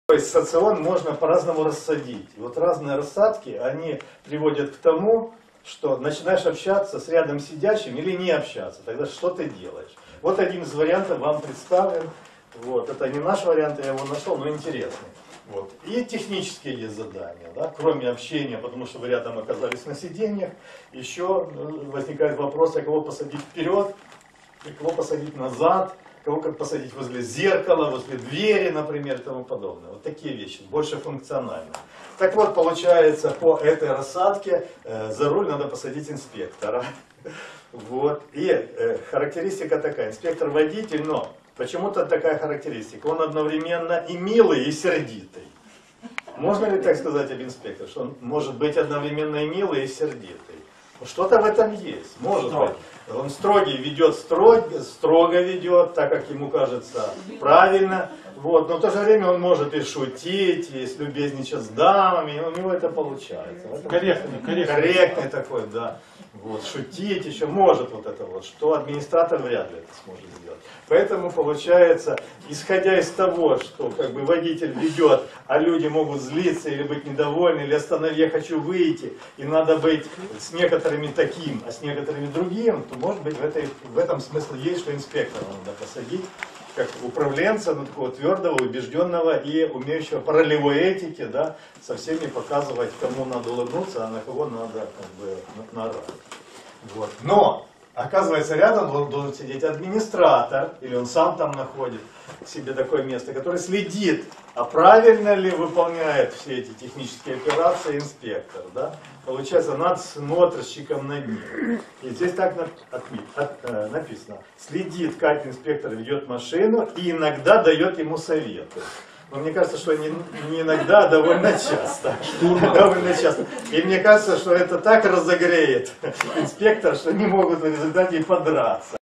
Все типы в автобусе. Отличия сценариев MBTI и Гуманитарной соционики Гуленко
Полное видео (16 мин) по этой теме вы можете заказать на Официальном сайте Если бы все типы социона ехали бы в одном автобусе и могли свободно выбрать себе место, где бы они сидели и чем занимались? Верны ли выводы MBTI? В этом видео соционик Виктор Гуленко рассказывает о подходе к описанию типов в MBTI и отличиях с Гуманитарной соционикой. Видео Официального сайта Гуманитарной соционики Гуленко.